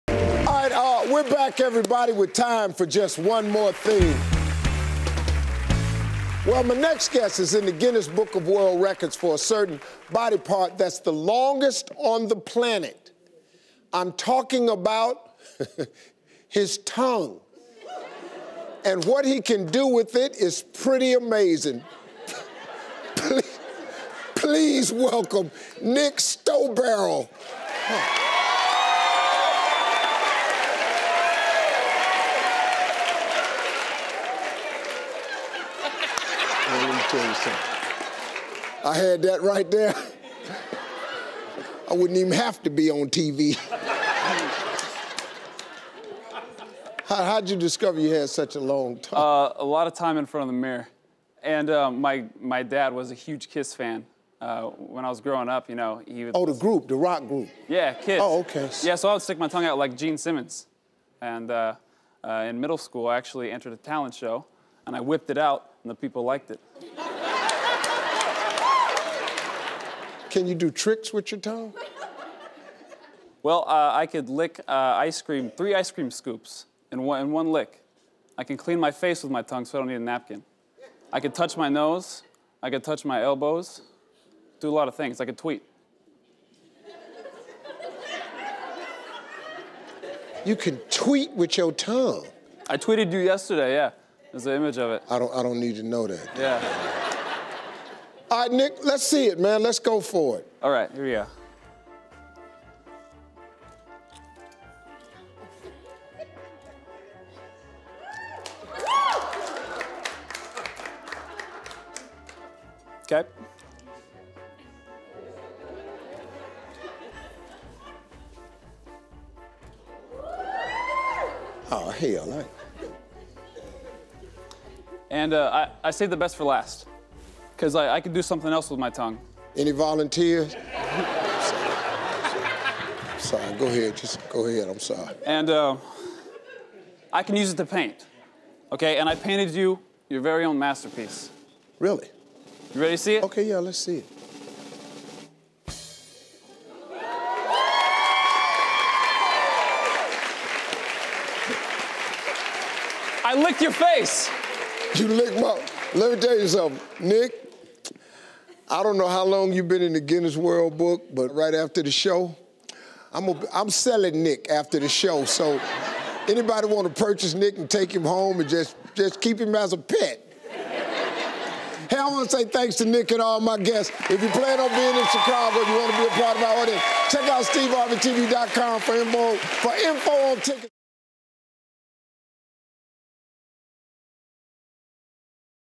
He's Got The Longest Tongue Ever!
This guest is in the Guinness Book of World Records for having one of the longest body parts on the planet. You have to see this to believe it!